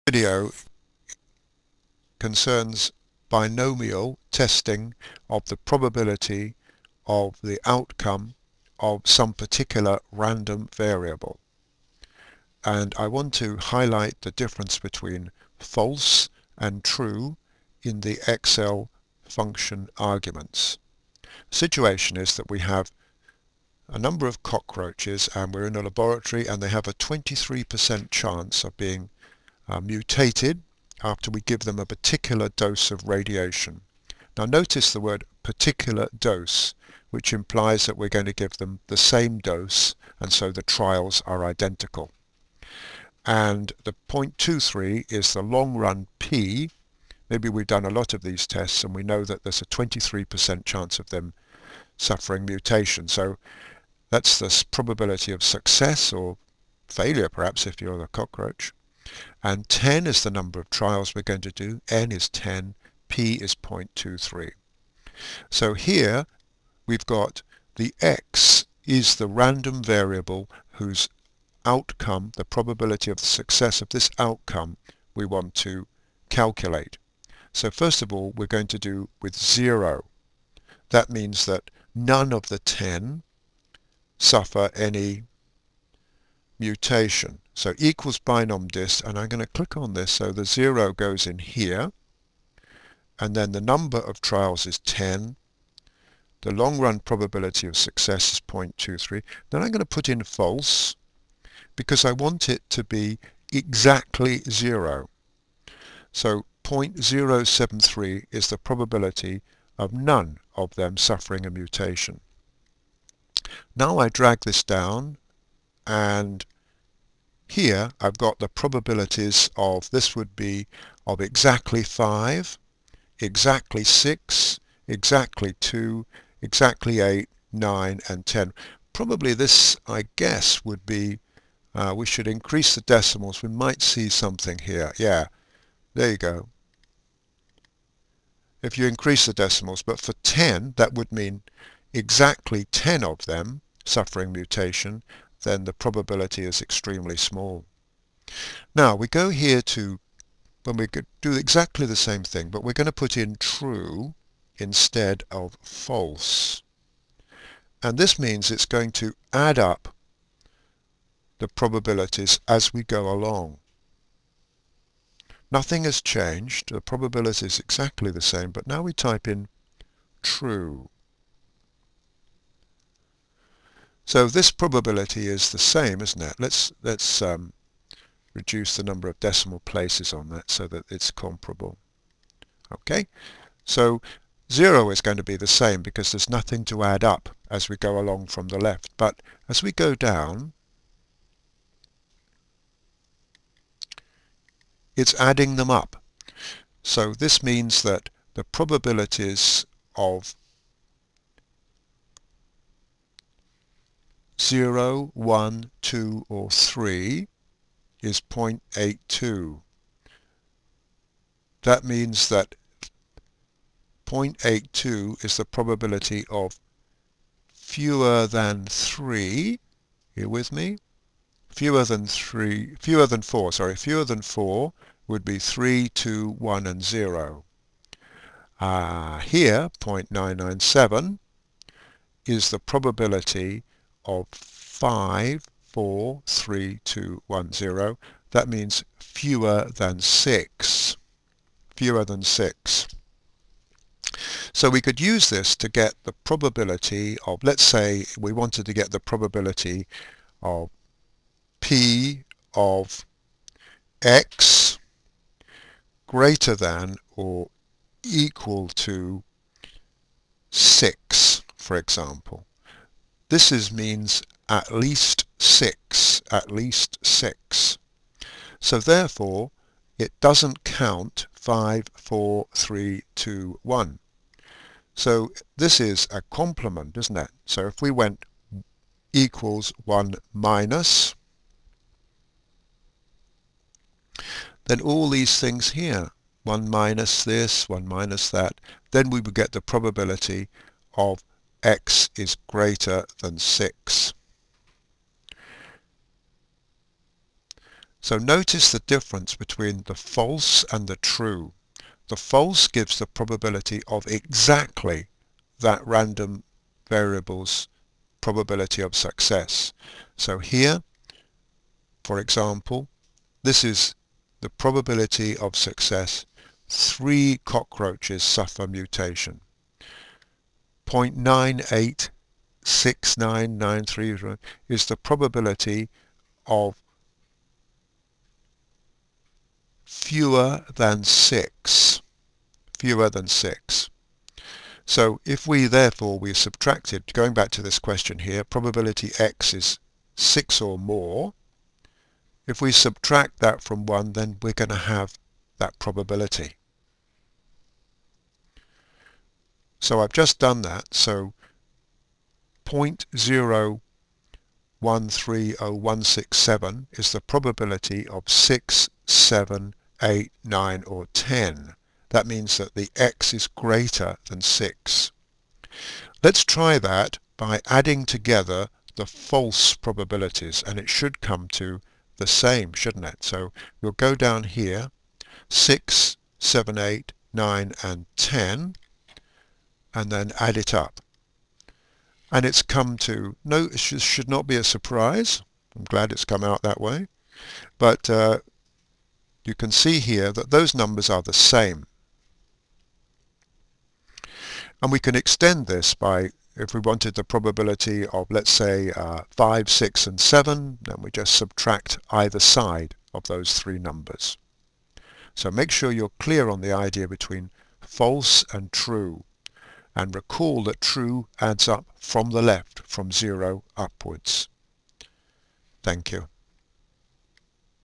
Binomials.avi
Binomials focussing on 'true' and 'false' statements in the function arguments in Excel.